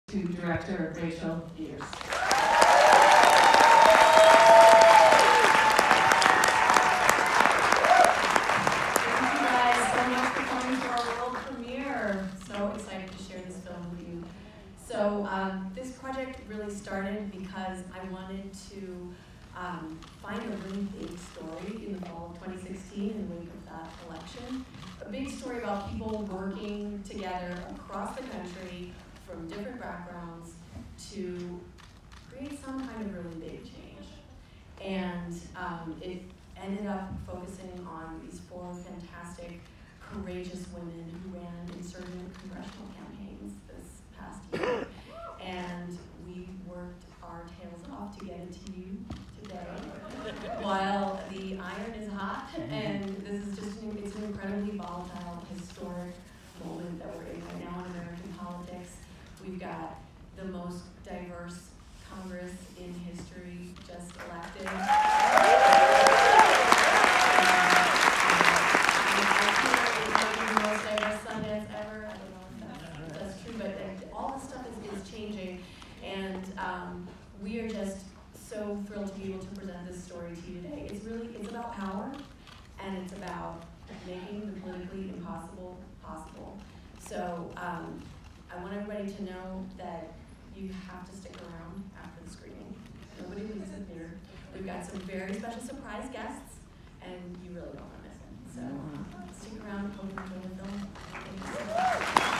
Knock Down The House director Rachel Lears introduces her Sundance Film Festival Premiere
Knock Down the House, which had its world premiere at the 2019 Sundance Film Festival, follows four women political candidates from around the country – a young bartender in the Bronx, a coal miner’s daughter in West Virginia, a grieving mother in Nevada and a registered nurse in Missouri – during the 2018 mid-term elections as they took to the campaign trail, built and engaged their bases, and built a movement. The film was directed by Rachel Lears and produced by Lears, Sarah Olson and Robin Blotnick.